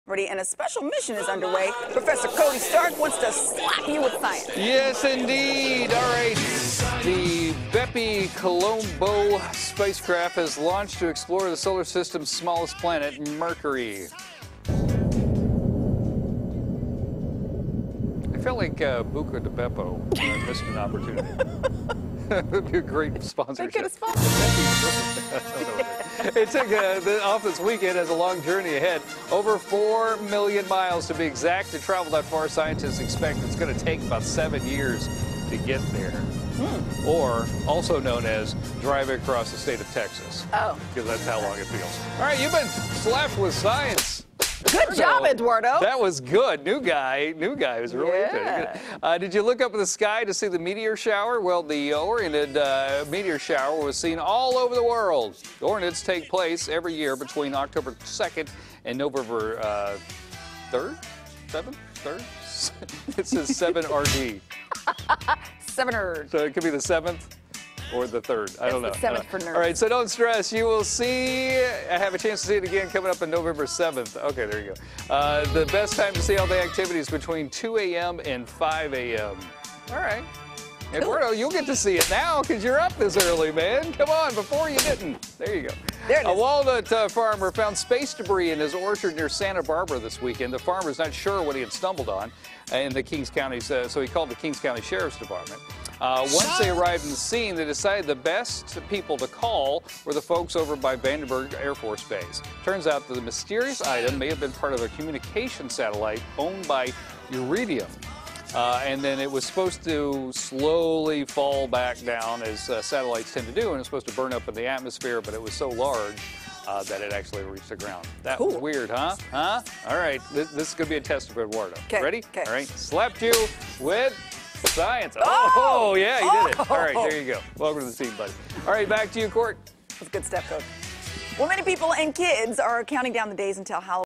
Slap You with Science: Space Junk Found
Cody is sharing some interesting science stories in today's "Slap you with Science!"